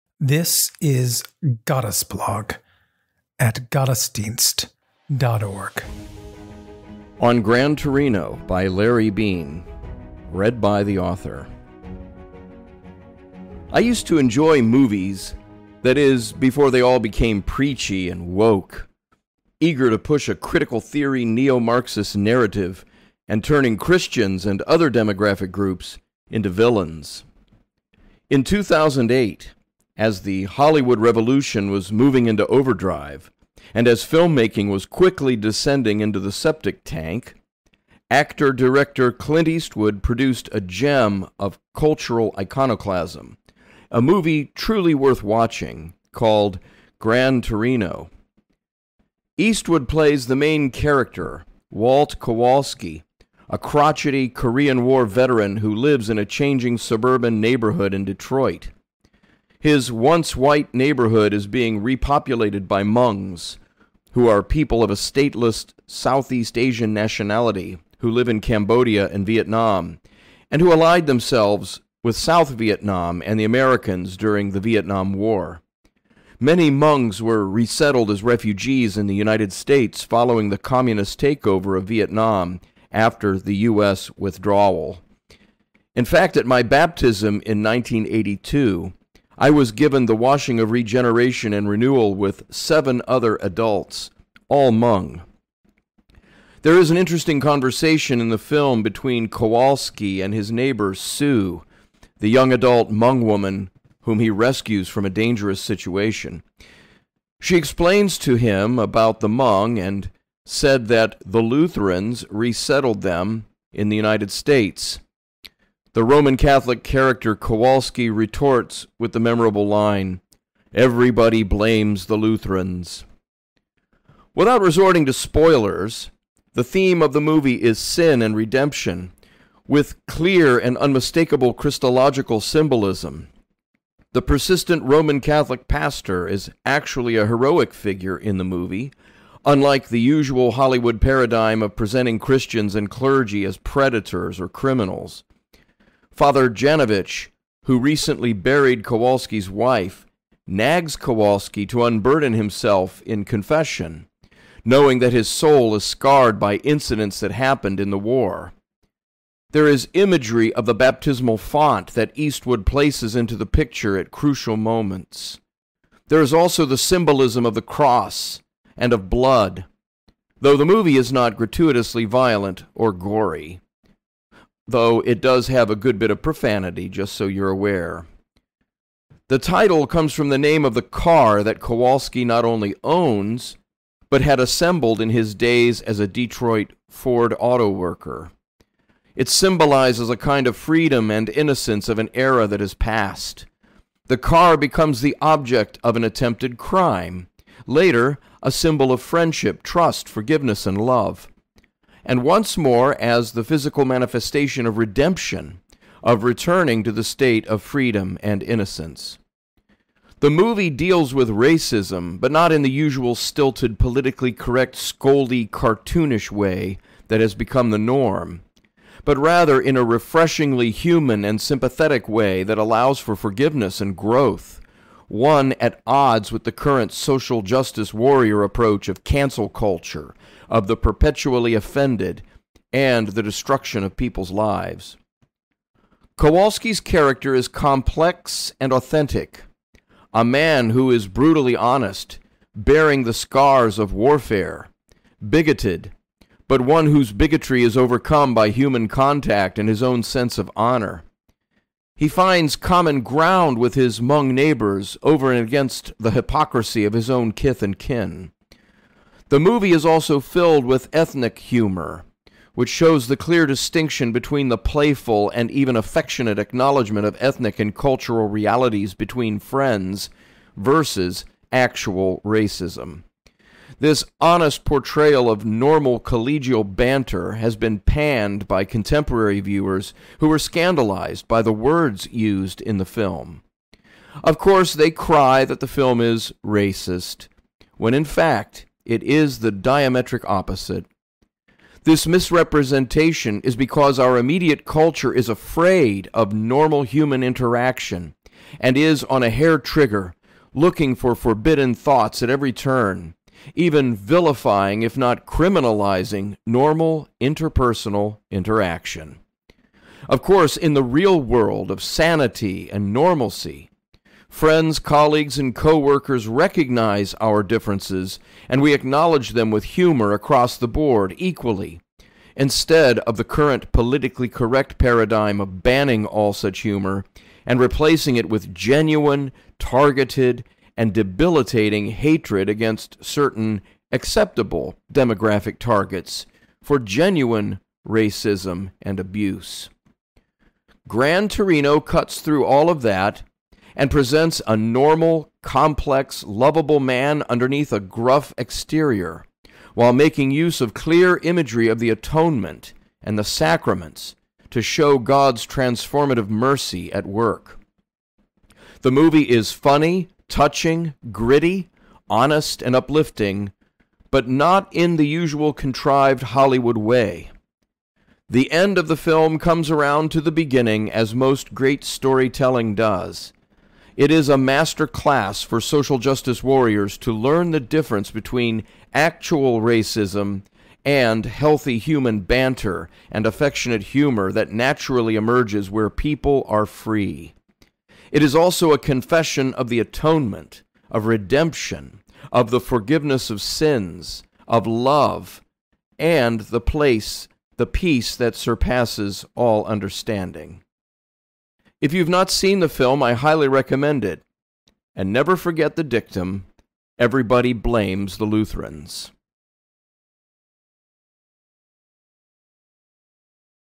[Gottesblog] On Gran Torino — Larry Beane
On Gran Torino

I used to enjoy movies, that is, before they all became preachy and “woke,” eager to push a Critical Theory Neo-Marxist narrative, and turning Christians and other demographic groups, into villains.
In 2008, as the Hollywood Revolution was moving into overdrive, and as filmmaking was quickly descending into the septic tank, actor/director Clint Eastwood produced a gem of cultural iconoclasm: a movie truly worth watching, called Gran Torino.
Eastwood plays the main character, Walt Kowalski, a crotchety Korean War veteran who lives in a changing suburban neighborhood in Detroit. His once-white neighborhood is being repopulated by Hmongs, who are people of a stateless Southeast Asian nationality who live in Cambodia and Vietnam, and who allied themselves with South Vietnam and the Americans during the Vietnam War. Many Hmongs were resettled as refugees in the United States following the communist takeover of Vietnam after the US withdrawal. In fact, at my baptism in 1982, I was given the washing of regeneration and renewal with seven other adults: all Hmong. There is an interesting conversation in the film between Kowalski and his neighbor Sue (the young adult Hmong woman whom he rescues from a dangerous situation). She explains to him about the Hmong, and said that “the Lutherans” resettled them in the United States. The Roman Catholic character Kowalski retorts with the memorable line: “Everybody blames the Lutherans.”Without resorting to spoilers, the theme of the movie is sin and redemption - with clear and unmistakable Christological symbolism. The persistent Roman Catholic pastor is actually a heroic figure in the movie, unlike the usual Hollywood paradigm of presenting Christians and clergy as predators or criminals. Father Janovich, who recently buried Kowalski’s wife, nags Kowalski to unburden himself in confession, knowing that his soul is scarred by incidents that happened in the war. There is imagery of the baptismal font that Eastwood places into the picture at crucial moments. There is also the symbolism of the cross and of blood - though the movie is not gratuitously violent or gory (though it does have a good bit of profanity, just so you're aware).
The title comes from the name of the car that Kowalski not only owns, but had assembled in his days as a Detroit Ford auto worker. It symbolizes a kind of freedom and innocence of an era that has passed. The car becomes the object of an attempted crime, later, a symbol of friendship, trust, forgiveness, and love - and once more as the physical manifestation of redemption, of returning to the state of freedom and innocence.
The movie deals with racism, but not in the usual stilted, politically-correct, scoldy, cartoonish way that has become the norm, but rather in a refreshingly human and sympathetic way that allows for forgiveness and growth - one at odds with the current Social Justice Warrior approach of cancel culture, of the perpetually-offended, and the destruction of people’s lives.
Kowalski’s character is complex and authentic: a man who is brutally honest, bearing the scars of warfare, bigoted, but one whose bigotry is overcome by human contact and his own sense of honor. He finds common ground with his Hmong neighbors over and against the hypocrisy of his own kith and kin. The movie is also filled with ethnic humor, which shows the clear distinction between the playful and even affectionate acknowledgement of ethnic and cultural realities between friends vs actual racism. This honest portrayal of normal collegial banter has been panned by contemporary viewers who are scandalized by the words used in the film. Of course, they cry that the film is “racist” - when in fact, it is the diametric opposite. This misrepresentation is because our immediate culture is afraid of normal, human interaction and is on a hair-trigger looking for forbidden thoughts at every turn, even vilifying, if not criminalizing, normal interpersonal interaction. Of course, in the real world of sanity and normalcy, friends, colleagues, and co-workers recognize our differences, and we acknowledge them with humor across the board, equally, instead of the current politically-correct paradigm of banning all such humor and replacing it with genuine, targeted, and debilitating hatred against certain “acceptable” demographic targets for genuine racism and abuse.
Gran Torino cuts through all of that, and presents a normal, complex, lovable man underneath a gruff exterior while making use of clear imagery of the atonement and the sacraments to show God’s transformative mercy at work.
The movie is funny, touching, gritty, honest, and uplifting, but not in the usual contrived Hollywood way. The end of the film comes around to the beginning, as most great storytelling does. It is a master class for Social Justice Warriors to learn the difference between actual ra